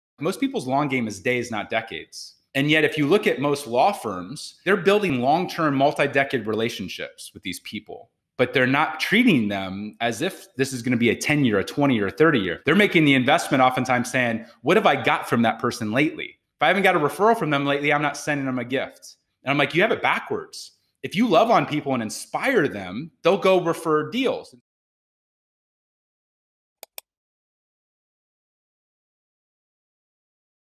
Ep. 43 — John Ruhlin — Playing the Long Game || Crisp Video
You must play the LONG GAME when it comes to building strong relationships with business partners and clients.

Learn how to better nurture your professional relationships through thoughtful gift giving by tuning in to the GCAPodcast with CEO and best-selling author of Giftology: John Ruhlin.